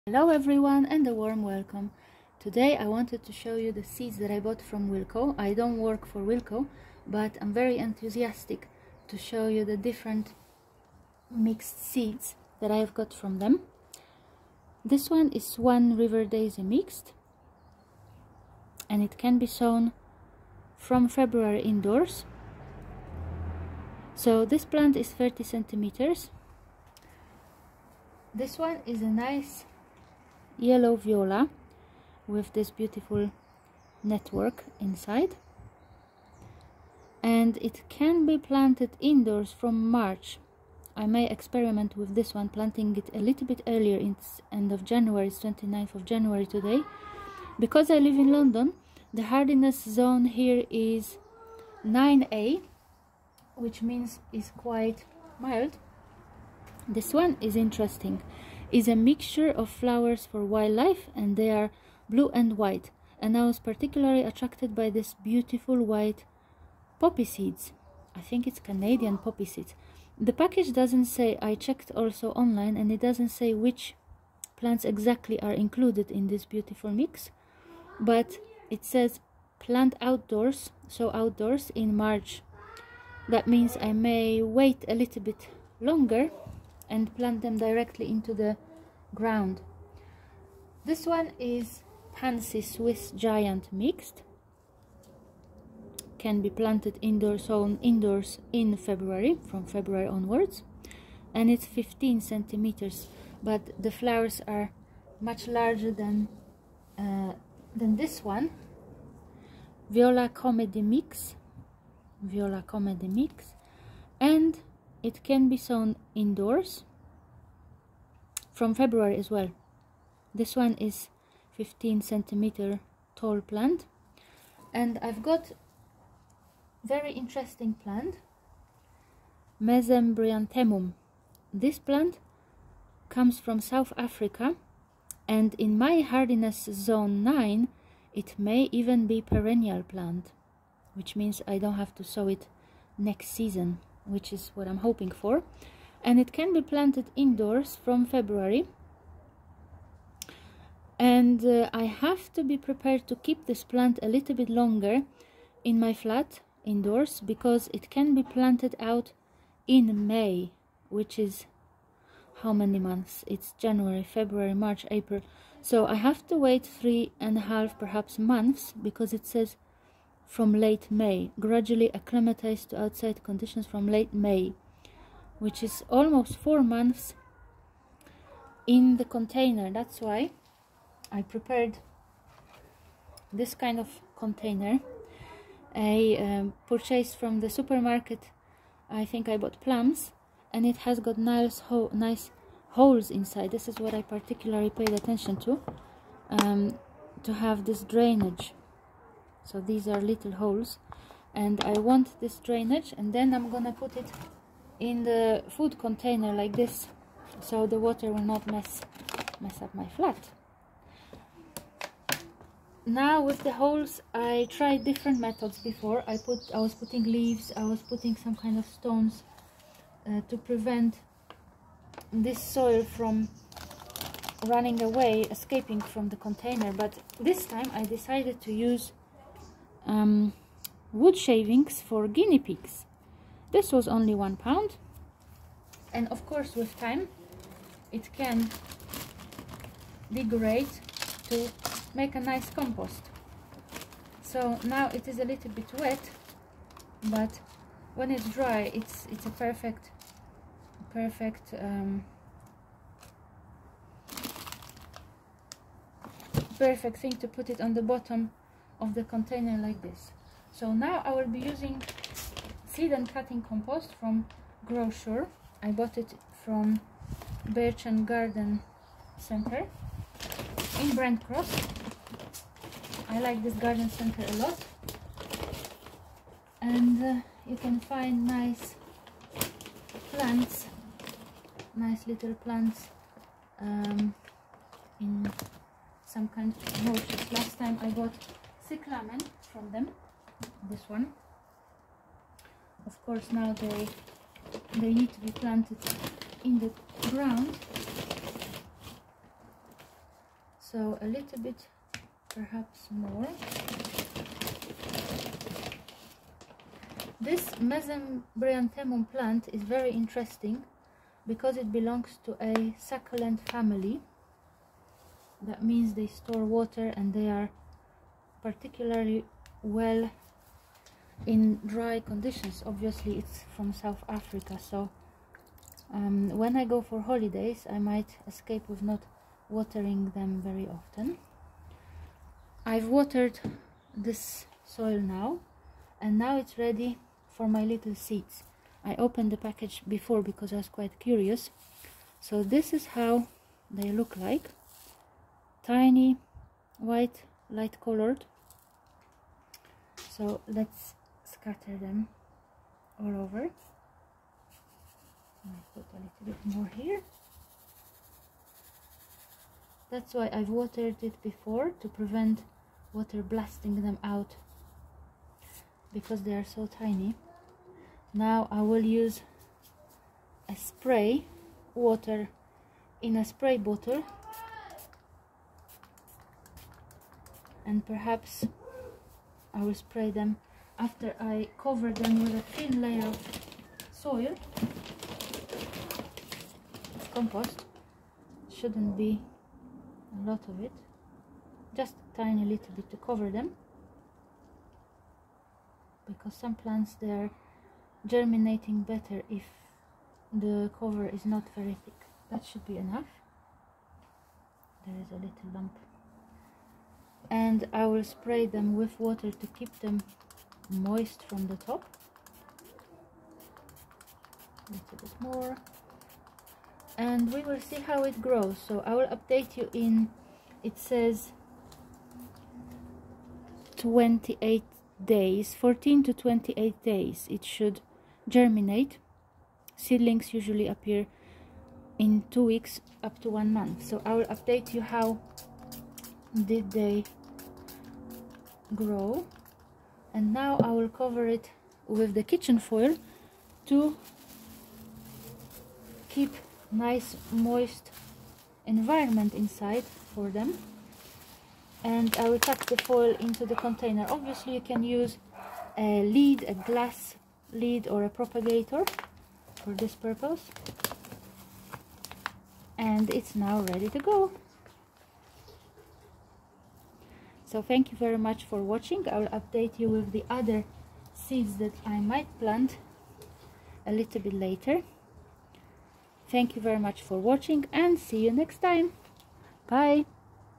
Spring seed mix from Wilko test , mesembryanthemum mix spring sowing
Various spring seeds sowing in late January into the indoor containers , South African succulent mesembryanthemum seeds